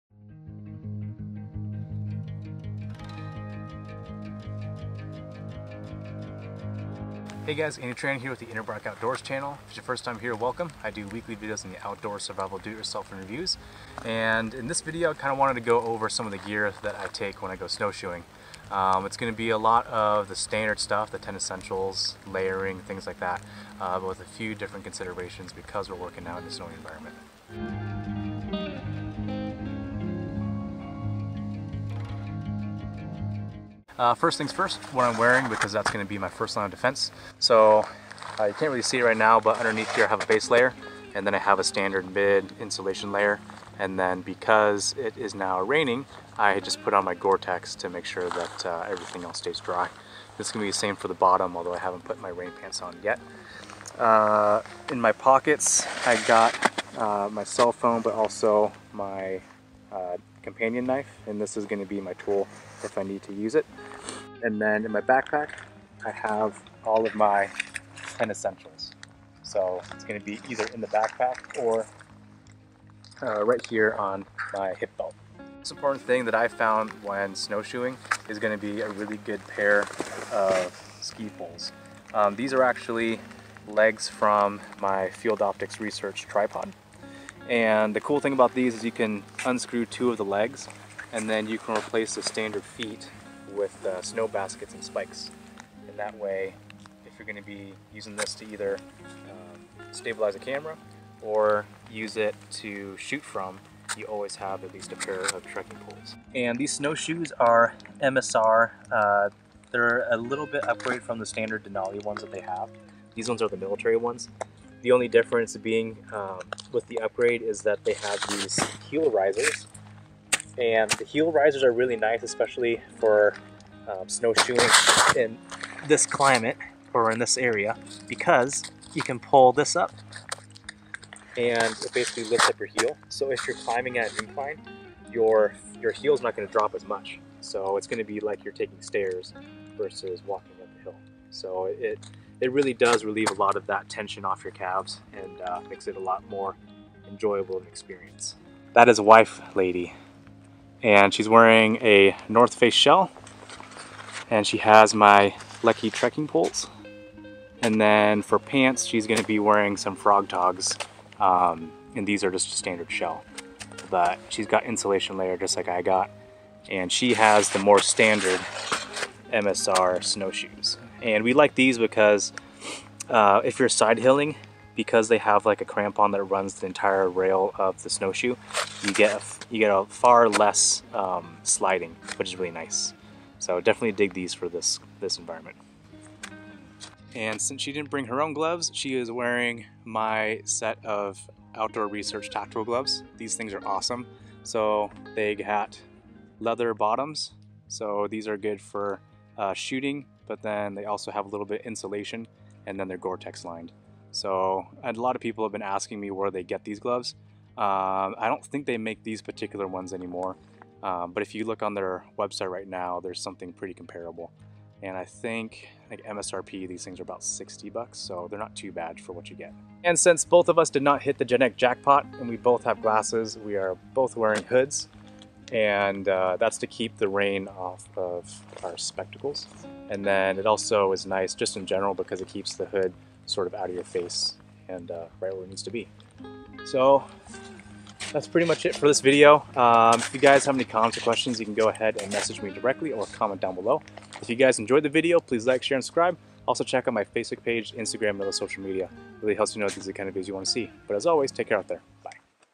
QUICK Guide to SNOWSHOEING Gear | Husband & Wife
The wife and I took a day snowshoeing hike up to an alpine lake and we decided to take you guys along to show you what kind of gear we wear/bring.